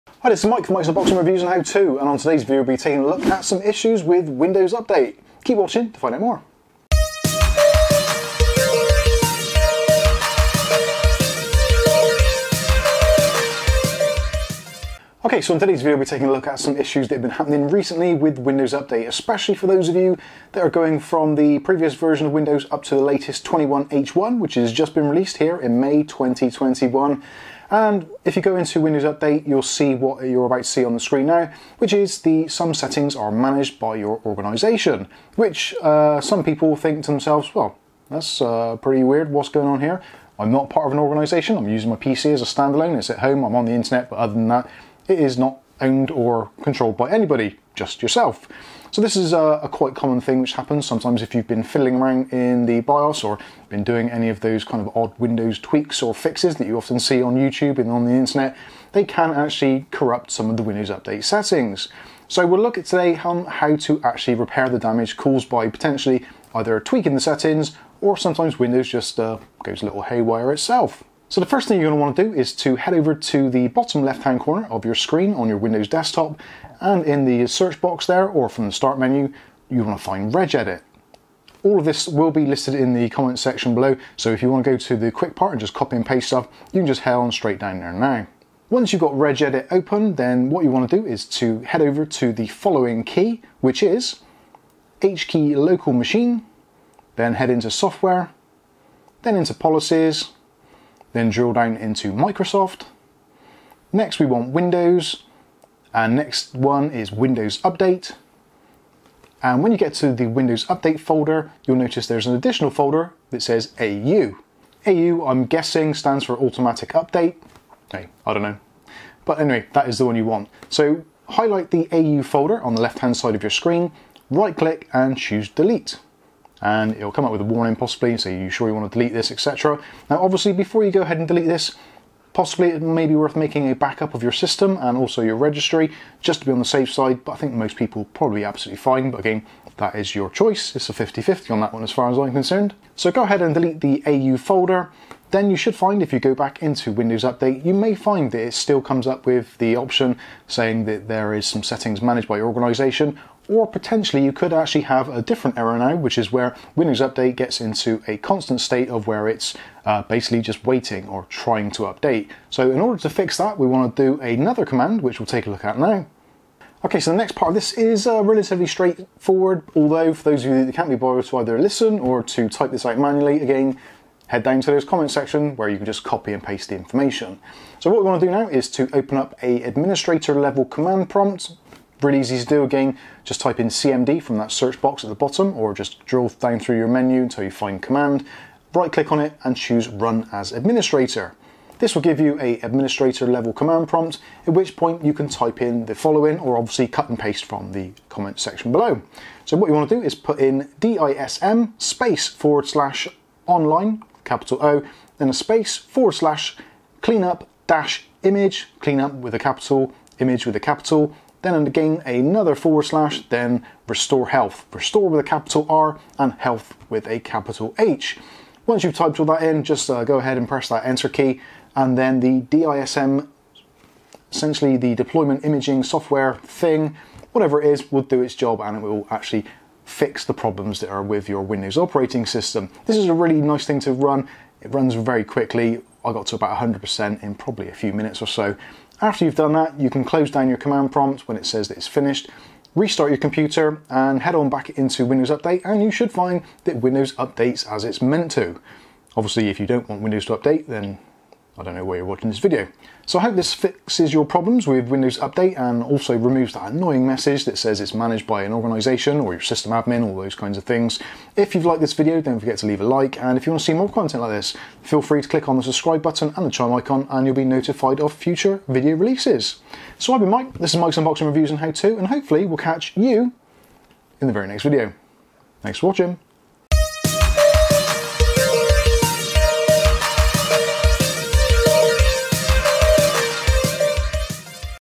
FIX Windows Update Error - Some Settings Are Managed By Your Organization Or Please Wait Loop
FIX Windows Update error
Some settings are managed by your organization or the dreaded Please Wait loop.

Windows 10 version 21H1 is causing some update issues and this also effects other versions so if you are finding either
Windows Update - Some settings are managed by your organization
or
Windows Update - Please wait (just stays on this forever)
Then this fix might help you.

BACKUP you system first or the Registry.

Open Regedit
locate this key
Computer\HKEY_LOCAL_MACHINE\SOFTWARE\Policies\Microsoft\Windows\WindowsUpdate\AU

delete the folder on the left called AU
then exit and reboot and the message should be gone.

If the please wait error is now a problem, open a admin level command prompt and copy and paste the following and then press enter

This should be done pretty quickly. When finished reboot and try Windows Updates again.